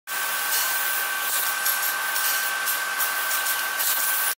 Fix Rivet On Wedge Pins of Ringlock Scaffold Ledgers - Wellmade Manufacturing Video
Fixing Rivets On Ringlock Wedge Pins
Wellmade Ring System Ledgers Manufacturing Video.
ISO 9001 & CE Certified Lead Manufacturer In China